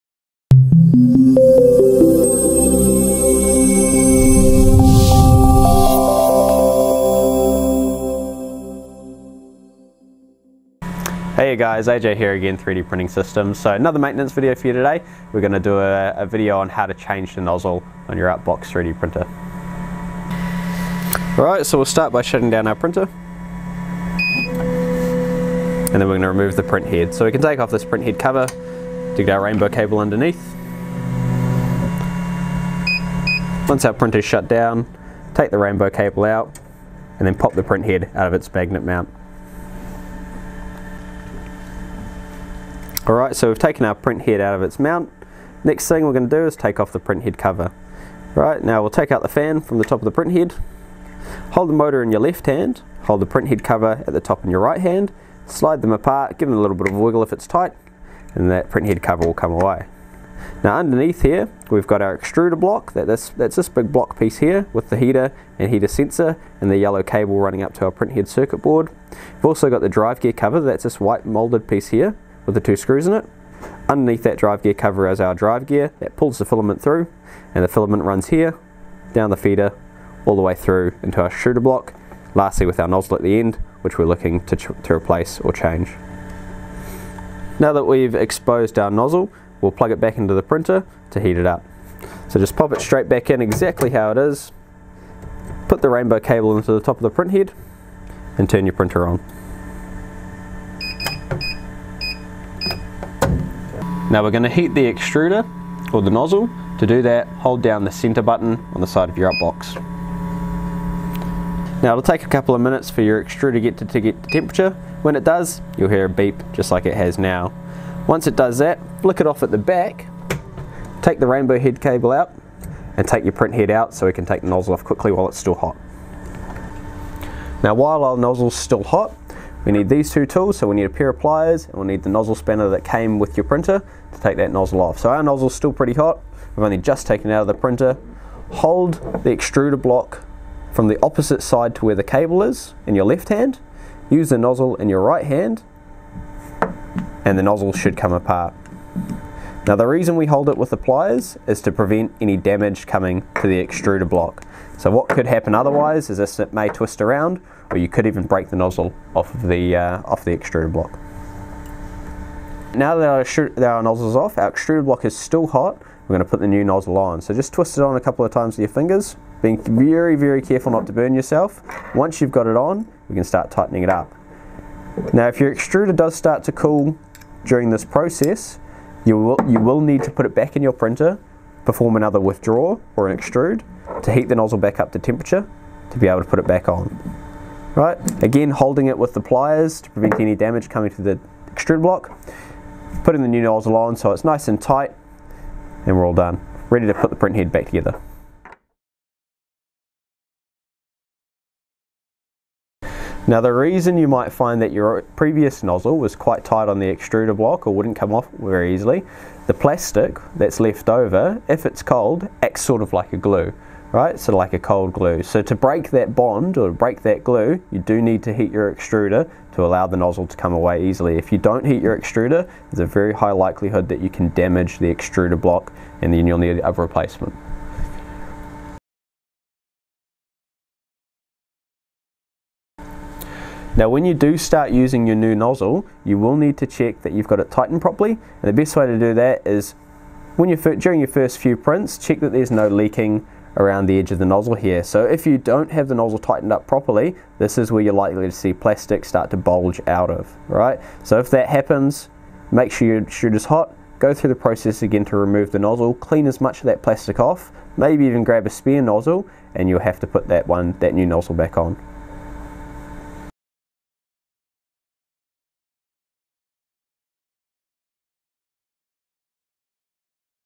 Support / UP Box / Changing the nozzle on your UP BOX 3D Printer
It's good to keep a few spare nozzles on hand to reduce printing downtime.

REMEMBER!! Always heat up the extruder block first before you attempt to remove the nozzle.